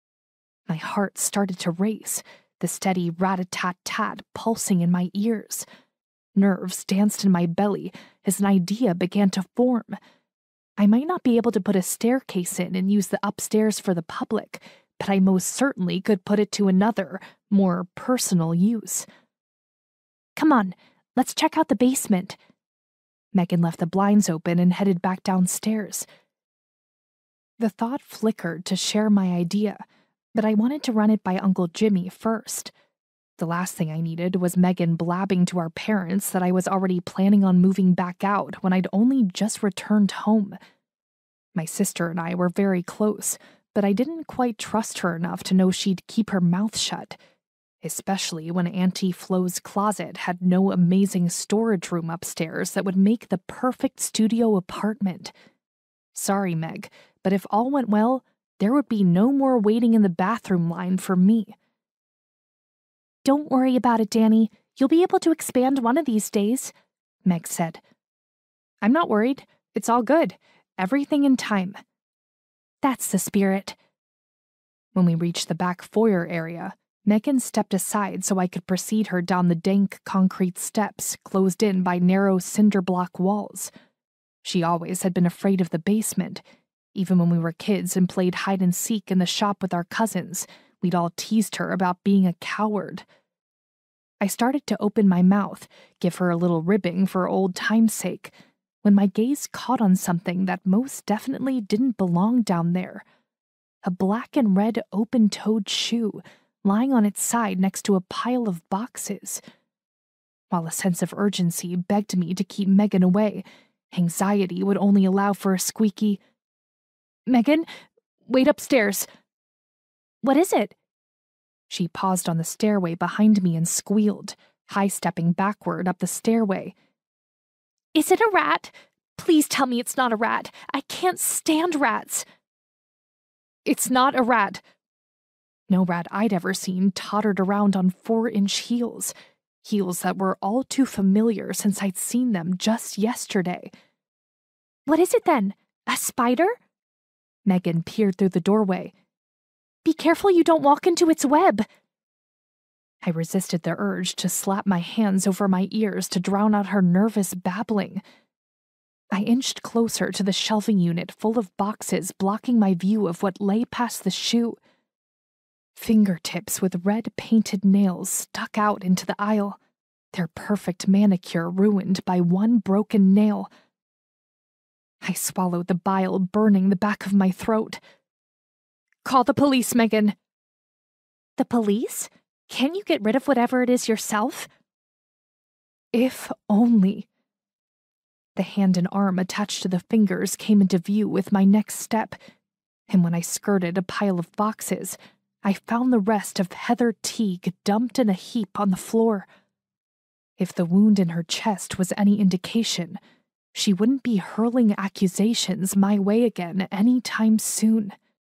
Audiobook Sample: Murder A La Mode
From author Lena Gregory comes a delicious new series that will warm your heart and leave you guessing until the very end . . .

When twenty-five-year-old Danika Delaney, black sheep of the Delaney clan, returns home to Long Island to take over Jimmie's, her eccentric uncle's old fashioned malt shop on eastern Long Island, she's not exactly thrilled. But things start to look up when her uncle tells her she can do whatever she'd like with the shop, and it seems she might realize her dream of a small trendy café. That is, until she discovers the body of her ex-boyfriend's estranged wife in a melted puddle of rocky road in the malt shop basement. With her two sidekicks, her sister and a good childhood friend, in tow, Dani searches for—or stumbles upon—one clue after another. But as she narrows down the suspect list, she realizes if she's not careful she may end up in a puddle of her own . . .